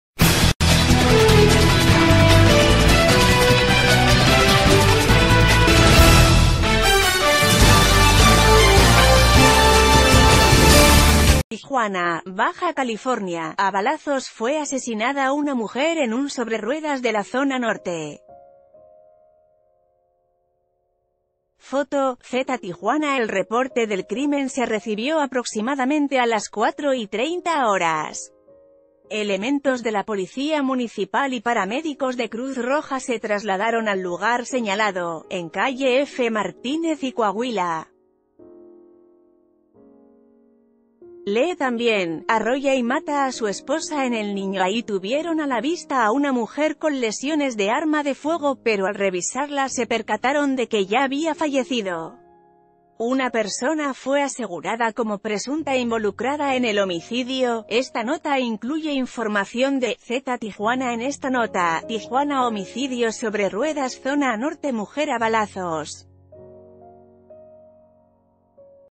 Ejecutan a mujer en "sobrerruedas" de Zona Norte
Tijuana, Baja California.- A balazos fue asesinada una mujer en un sobrerruedas de la Zona Norte. Foto: Zeta Tijuana El reporte del crimen se recibió aproximadamente a las 16:30 horas. Elementos de la Policía Municipal y paramédicos de Cruz Roja se trasladaron al lugar señalado, en calle F. Martínez...